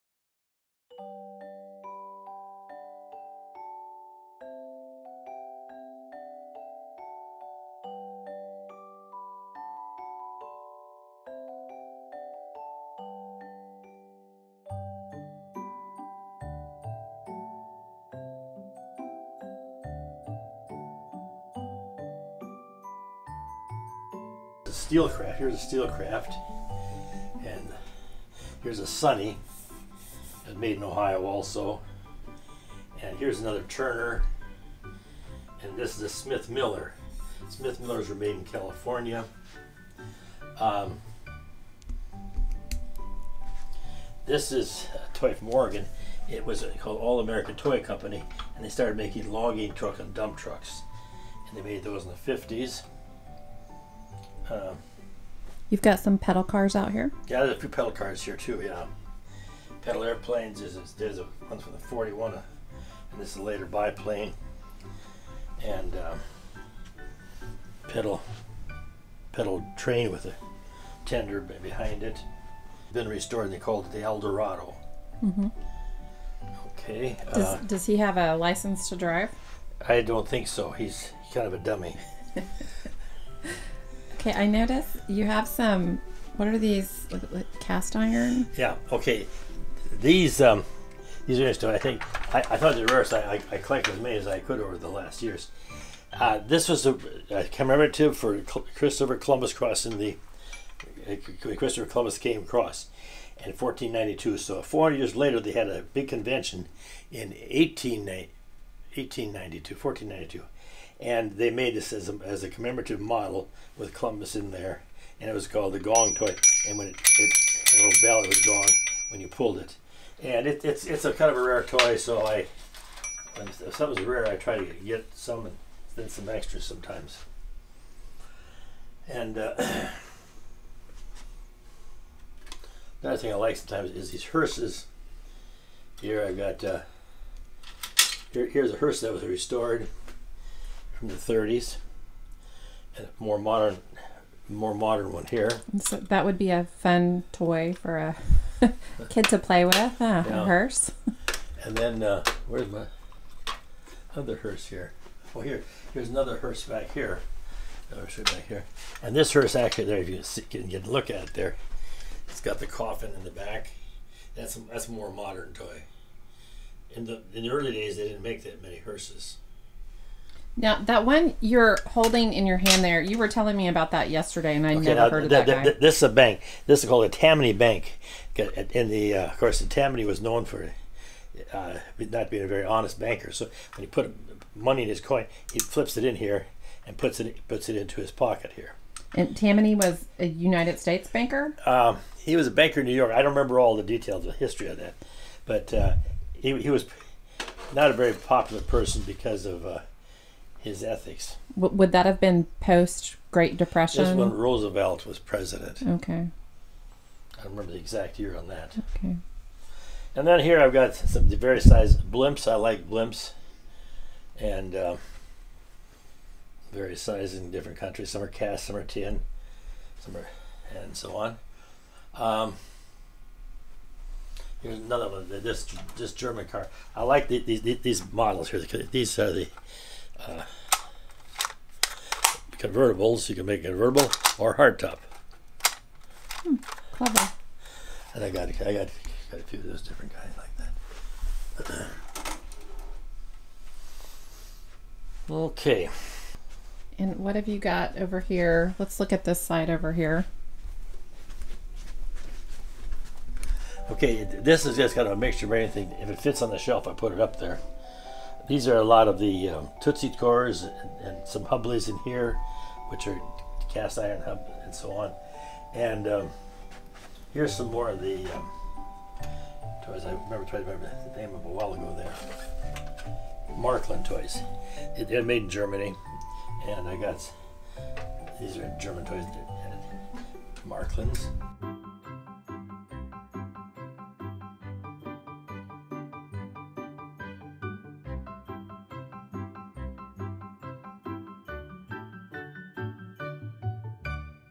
Don's Toy Room Tour Part 1
Don is an antique toy collector with THOUSANDS  of toys!  Join us for a tour of Don's toy room.